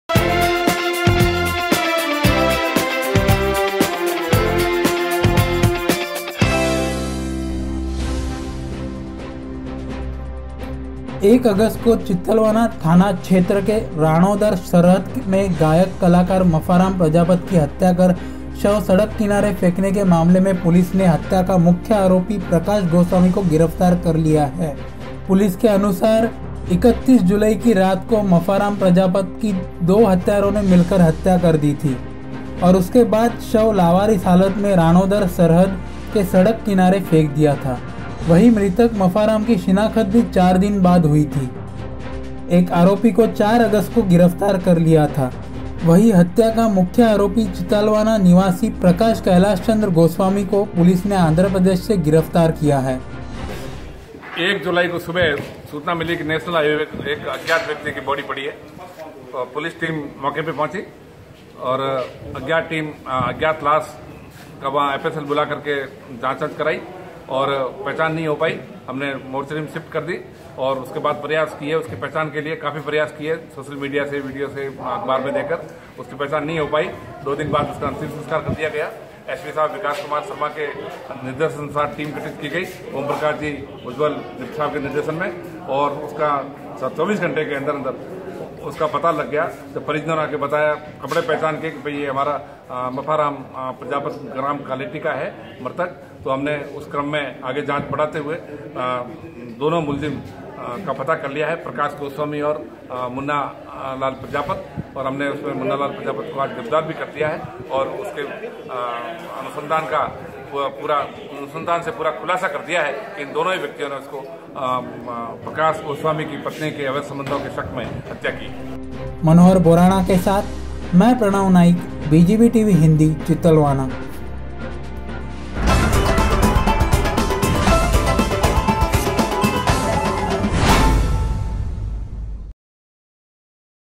चितलवाना- गायक कलाकार मफाराम प्रजापत की हत्या का मुख्य आरोपी गिरफ्तार।
BGB Tv Hindi | Special Report | 11 August 2018 |

V/O -  Pranav Naik

Reporter -  Manohar Borana

Editor - Poonam Maurya

published By - Poonam Maurya

अभी आप बनो आपके  विभाग के पत्रकार

हम दिखाएंगे आपकी खबर अपने BGB Tv पर